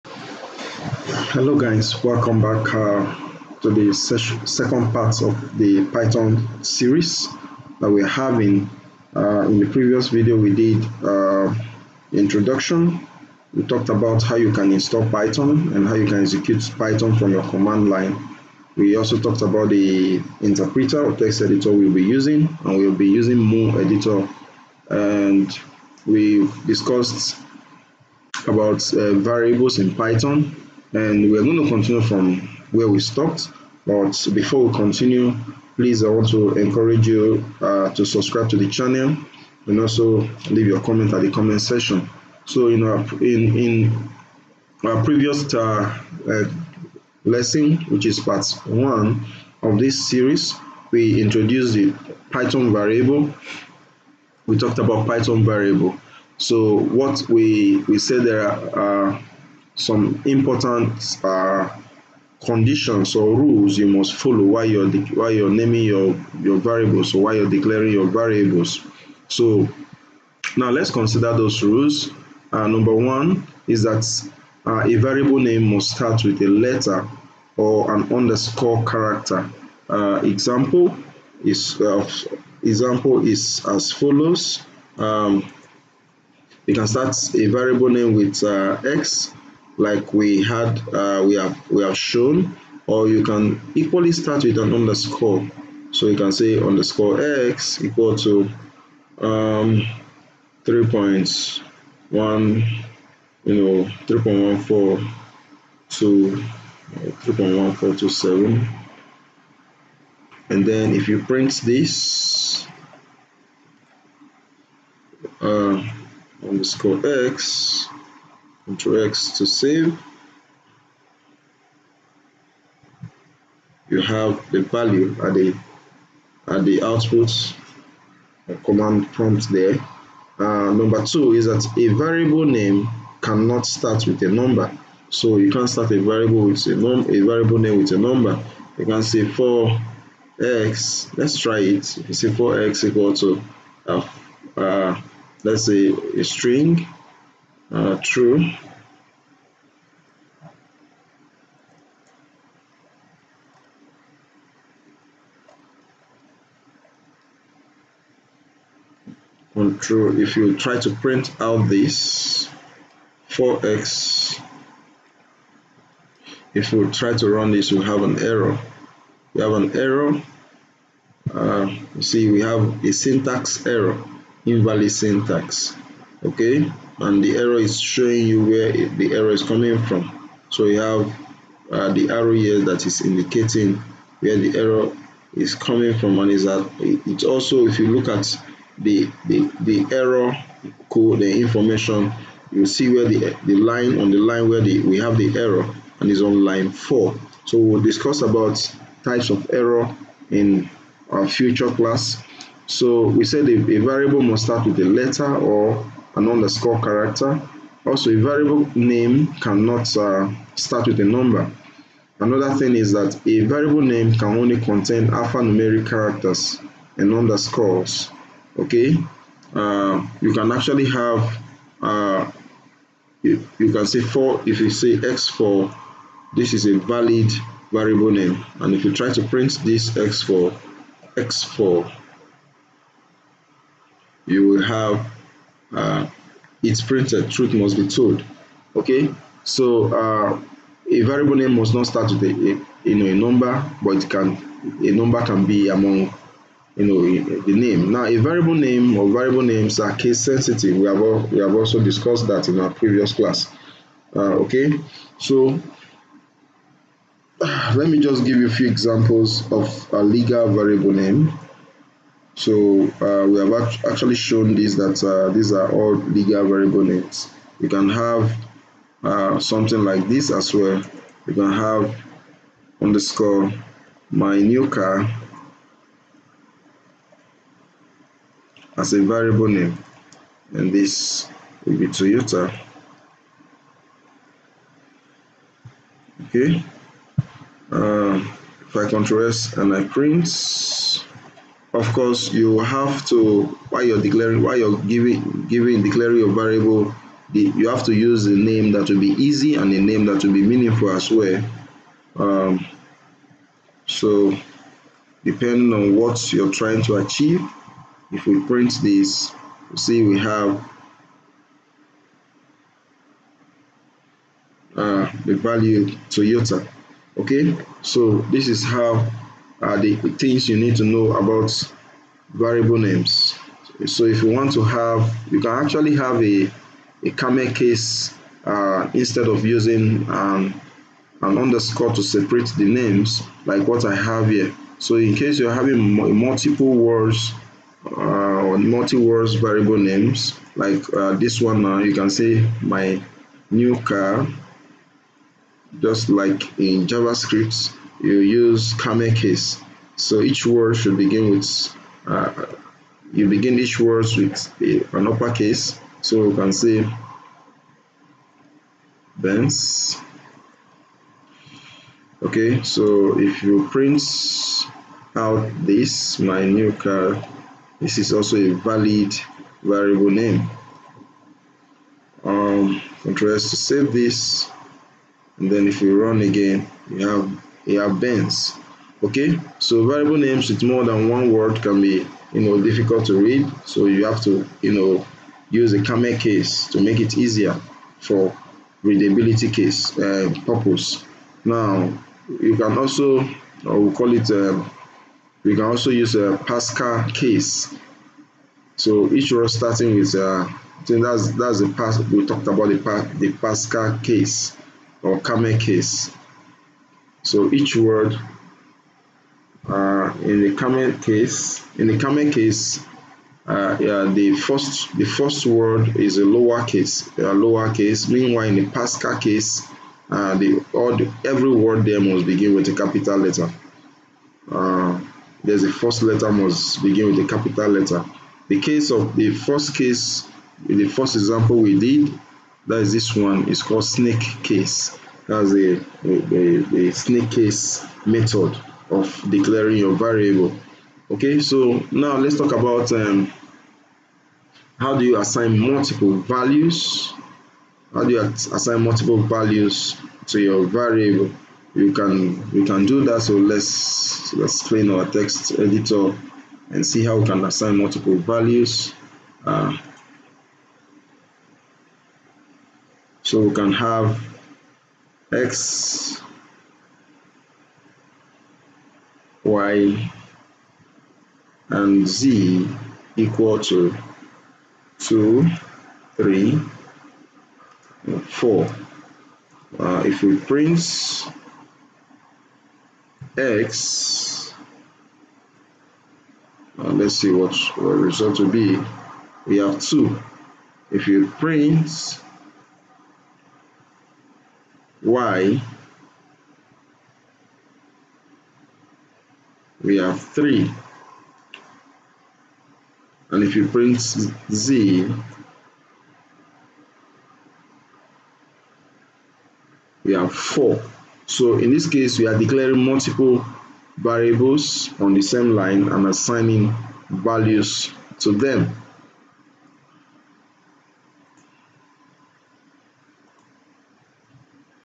Python for Everyone - Introduction to Python 3 Variables Part-2
In this video, we will be looking at the basics of Python 3, starting from scratch. We will look at the introduction to Python 3, What is Python used for, Introduction to Python Variables, Python Variable Declaration, Comments in Python, Python Command Line, etc.